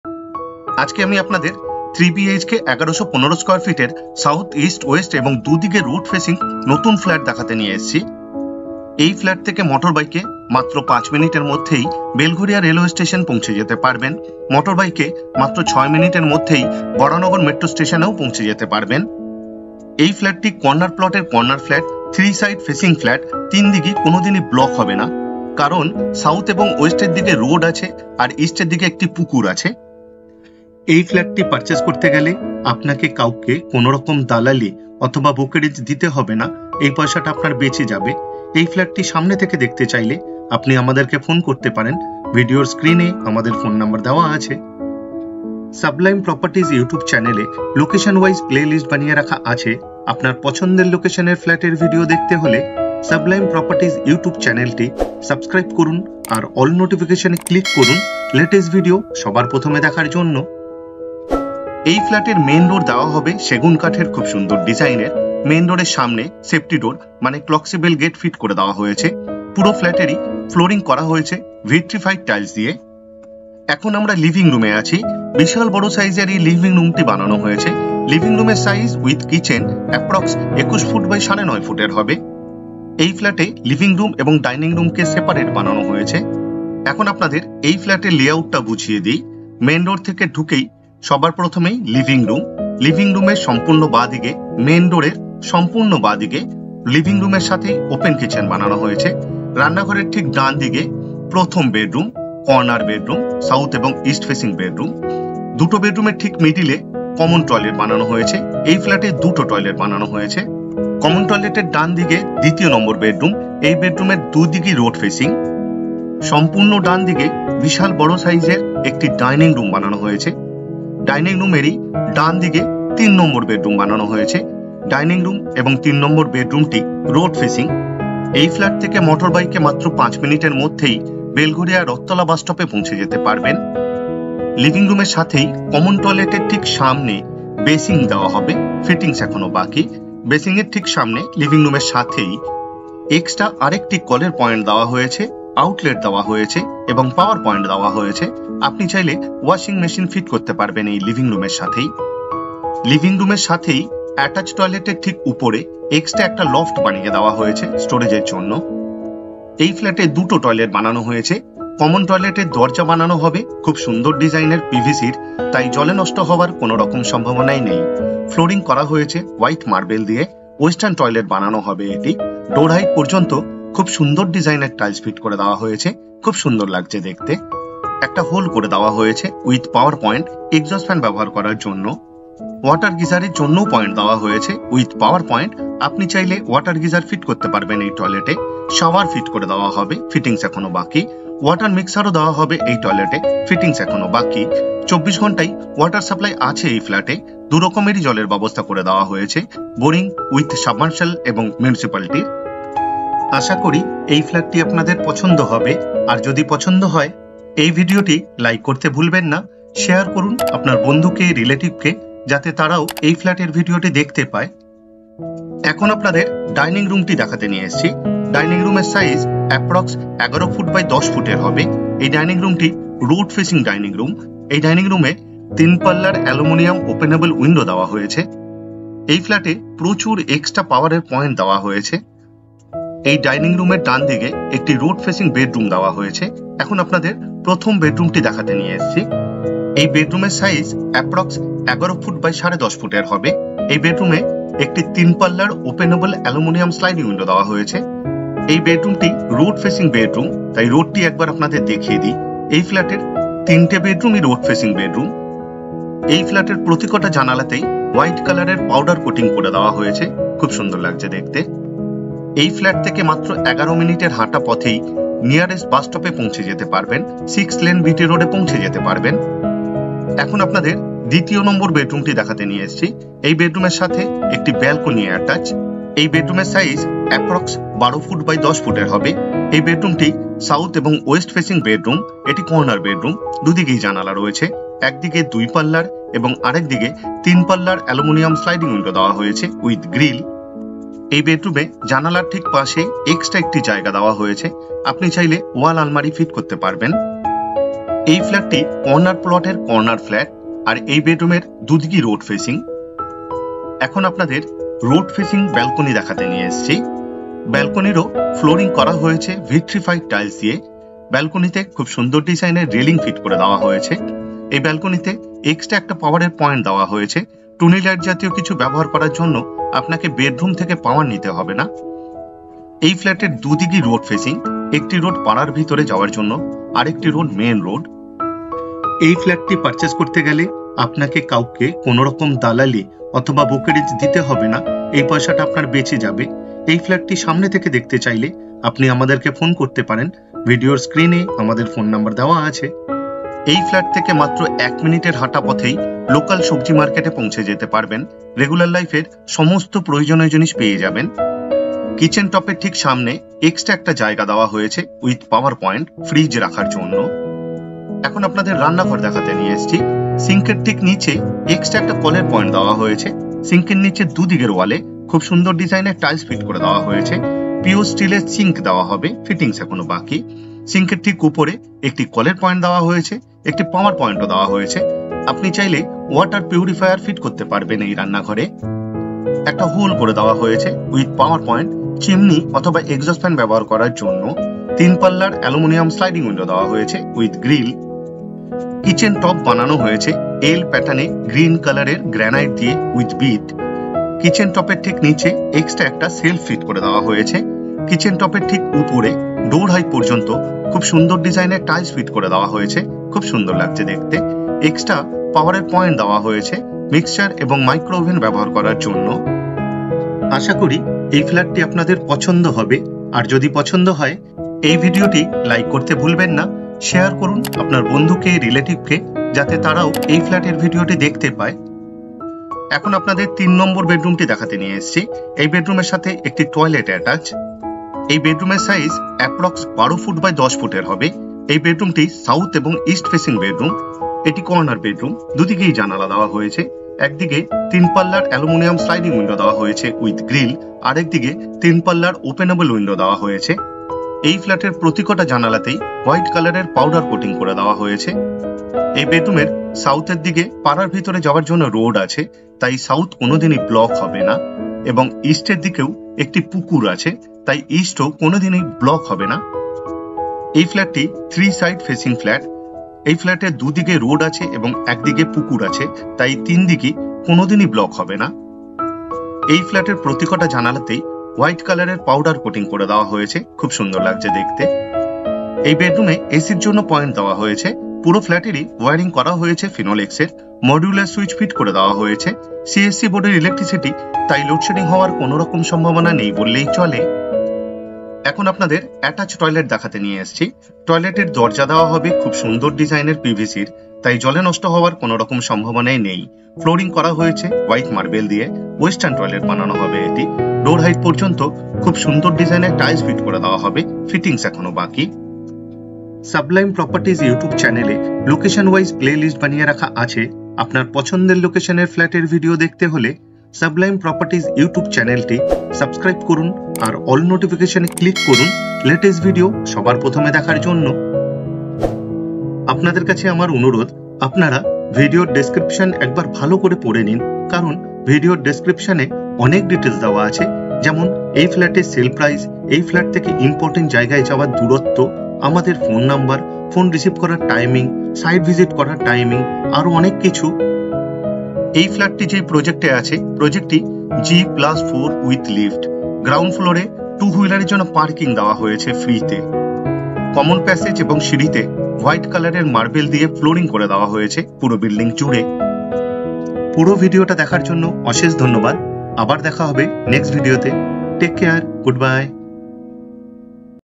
Belgharia | 3BHK 1115sqft SOLD Only 40.34 Lakhs New Flat Near Belgharia Station
Available Property for Sale Playlist Link 👇🏼

Property details:
✅️ Buy this Flat without Brokerage.
✅️ 3BHK Ready to Move New Flat.
✅️ 1115sqft Super Buildup.
✅️ 3 Bedroom, 2 Toilet, 1 Kitchen, 1 Dining Room & 1 Living Room.
✅️ South, East, West & 2 Side Road Facing.
✅️ Only 4th Floor (SOLD).
✅️ 40.34 Lakhs Fixed Price.
✅️ Loanable Property.
✅️ G+4 Stand Alone Building with Lift.
✅️ Two Wheeler Parking Free.
✅️ 5mins from Belgharia Railway Station by Motorbike.

Material used:
1) Room & Balcony Flooring = Vitrified.
2) Toilet Flooring = Marbel,
3) Switch = Modular,
4) Wiring = Finolex,
5) Electricity = CESC Ltd.,
9) AC Point = 1 Room
7) Main Door = Wooden (Segun),
8) Window = Aluminium Openable,
9) Sanitary Ware = Jaguar Fittings,
10) Water = 24 Hours (Boring & Municipal),

Approx Distance by Motorbike from this Flat:
3 minutes / 850 Mtrs distance from B. T. Road (Bus Stop),
4 minutes / 1.4 KM distance from Belgharia Railway Station,
5 minutes / 1.5 KM distance from Rathtala Traffice More,
6 minutes / 2.3 KM distance from Baranagar Metro Station,
20 minutes / 9.7 KM distance from Dum Dum Airport (2no Gate),
35 minutes / 12 KM distance from Sealdah Railway Station,
40 minutes / 14 KM distance from Howrah Railway Station.
40 minutes / 14 KM distance from Esplanade Bus Terminal..

Sublime Properties

Buy Property without Brokerage.

Site Visit Time: 10:00 am to 5:00 pm.

Please call us between 10:00 am to 7:00 pm (Indian Time)